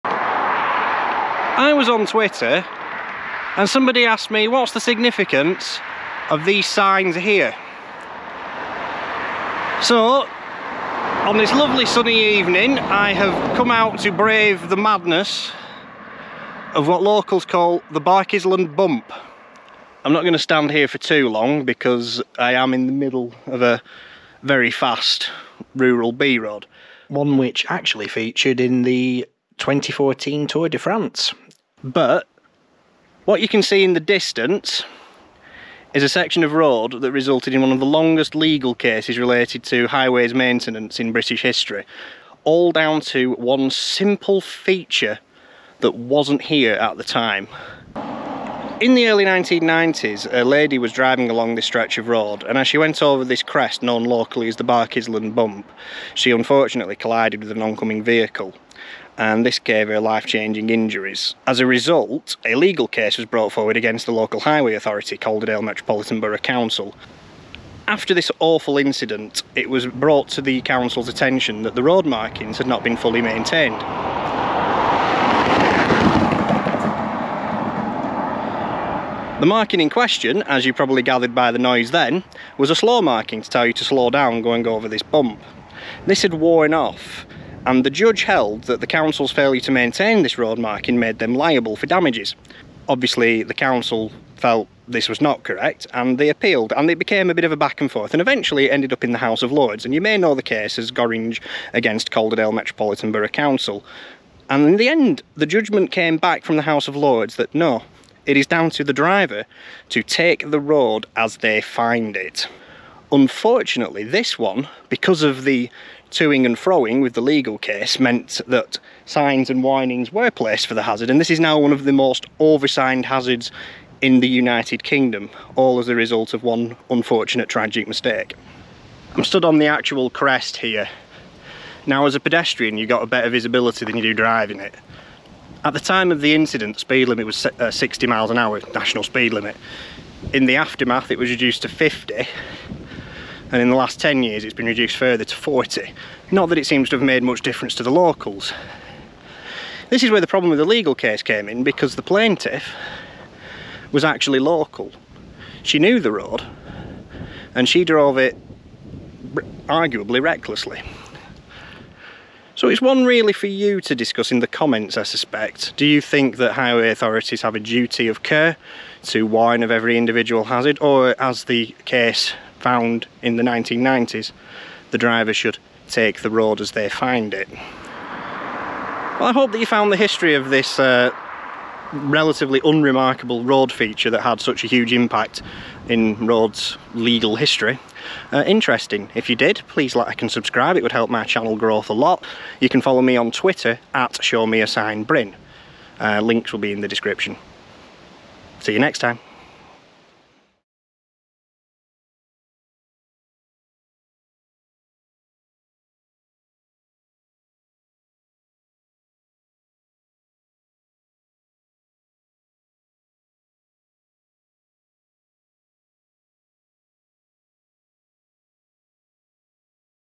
The Tiny Bump That Made A Big Legal Mess!
The Barkisland Bump on the B6113 is pretty unremarkable, apart from it being the location of an event which had huge repercussions for English highway law.

Maps collated to illustrate this video are available